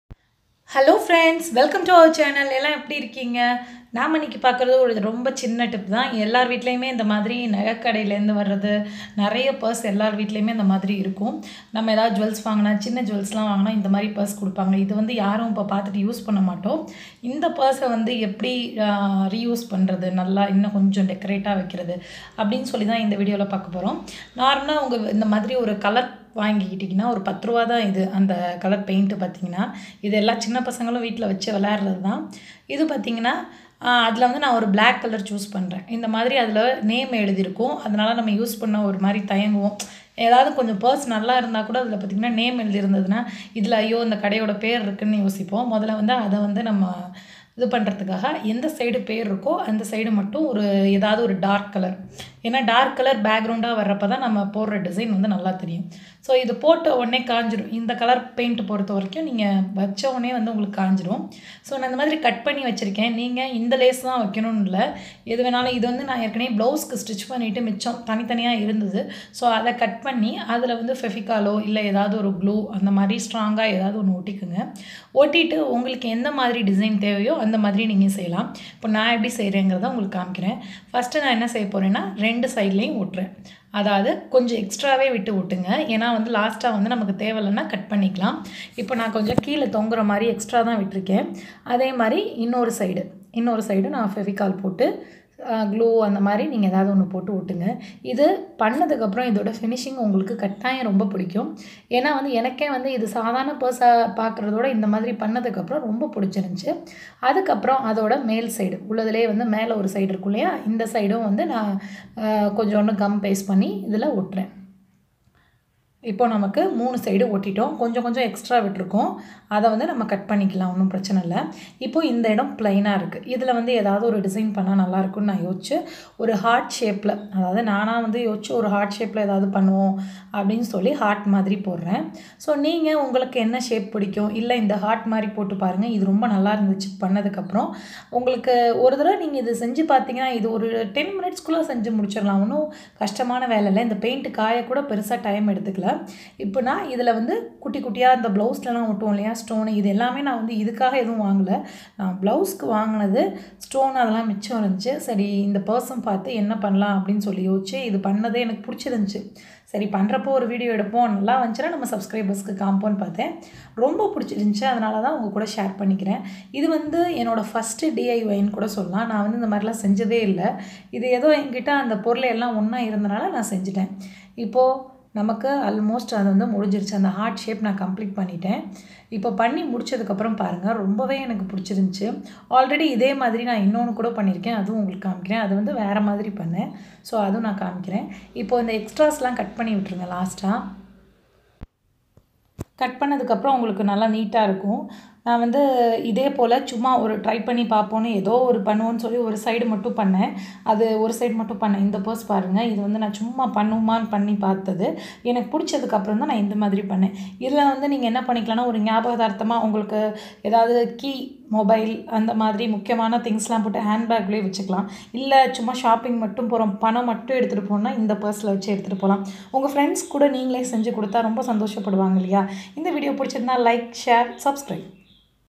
நகை கடையில் தரும் பர்சை இப்படி செய்து பாருங்கள் செம்ம |Diy |my first DIY /BUDS 2 BLOOM
homemade floor cleaner without any chemicals